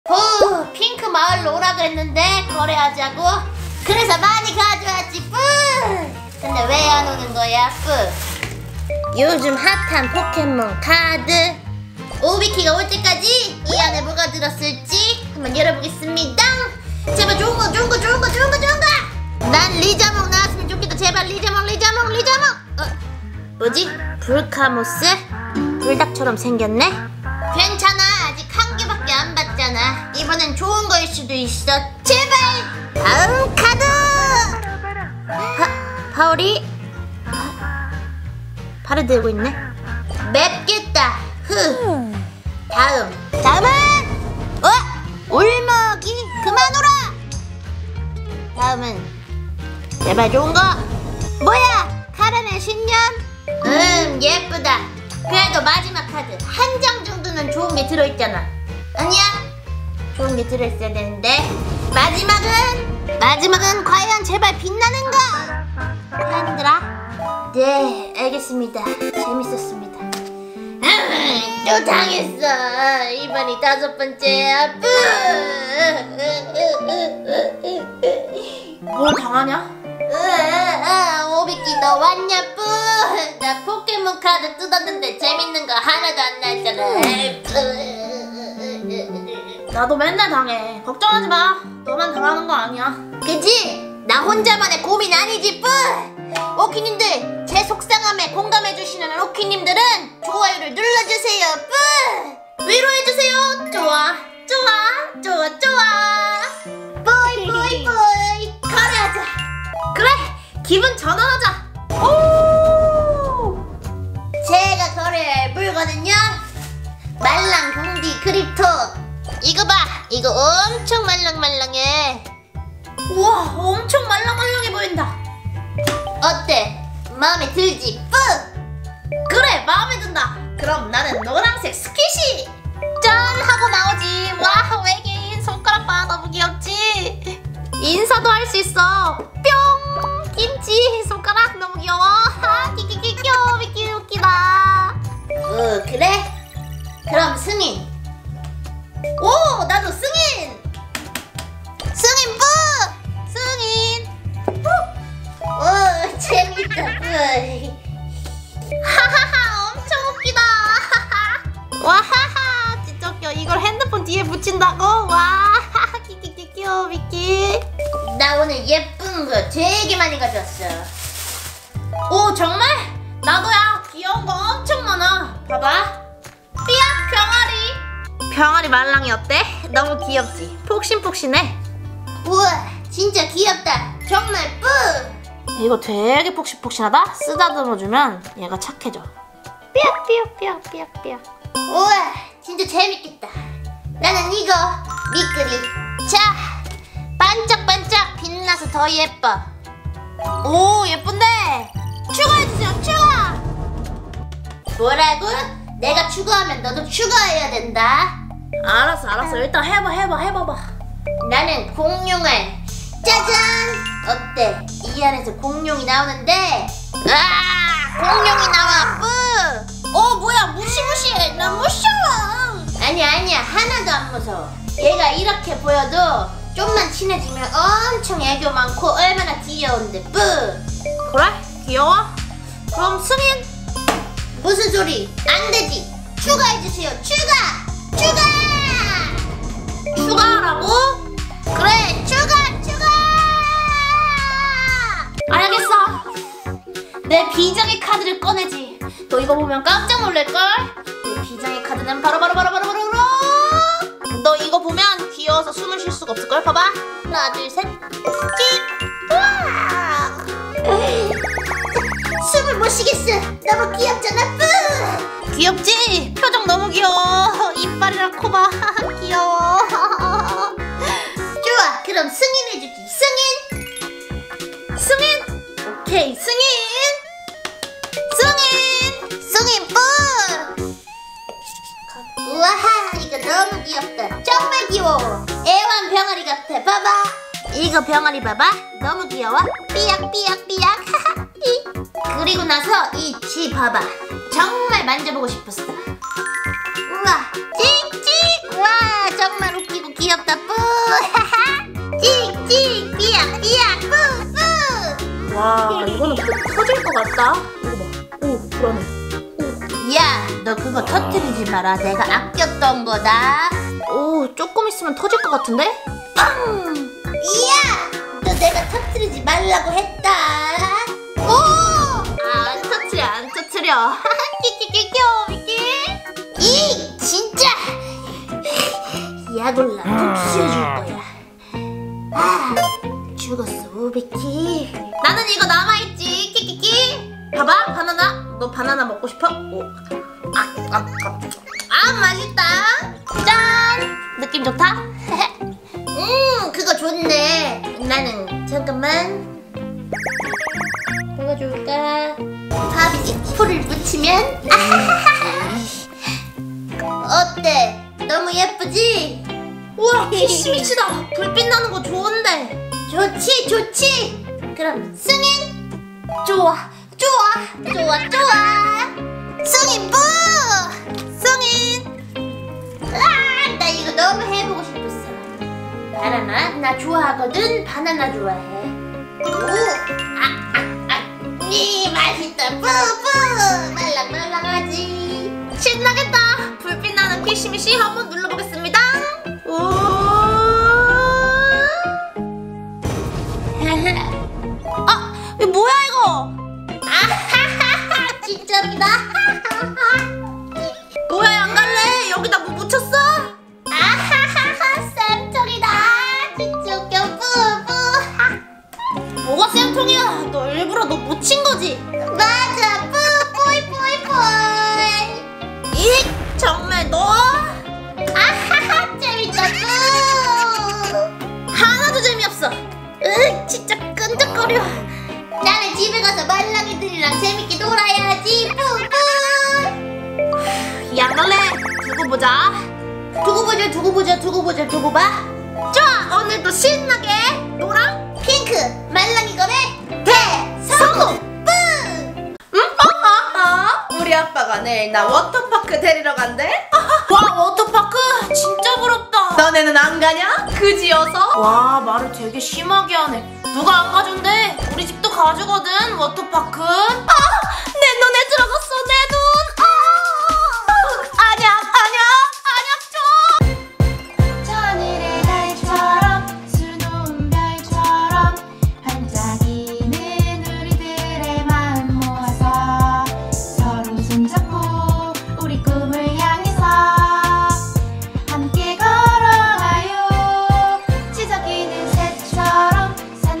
핑크VS노랑 말랑이 거래하다가 장난치기 [사이다 복수]
양갈래 핑크마을에 가서 오비키를 기다리는 동안
포켓몬 카드를 열어보는데
과연 좋은 게 들어있을까요?